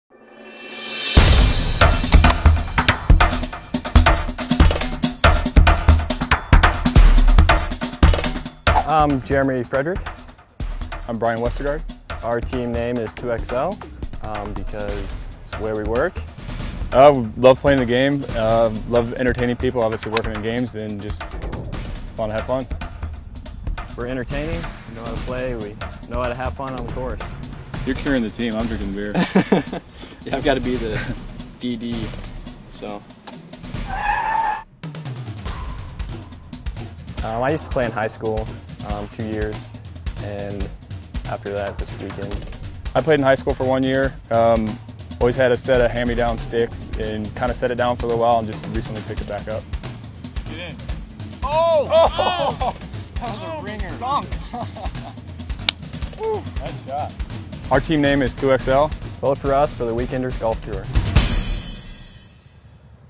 Meet Team 2-XL
On August 21, 2006, these twelve teams auditioned and competed in 116 degree weather for the right to become a "Weekender".

Your vote will help decide who proceeds to the Pilot Episode, and who stays home.  Click on the team picture to view their audition tape, then click below to vote for your favorite THREE teams!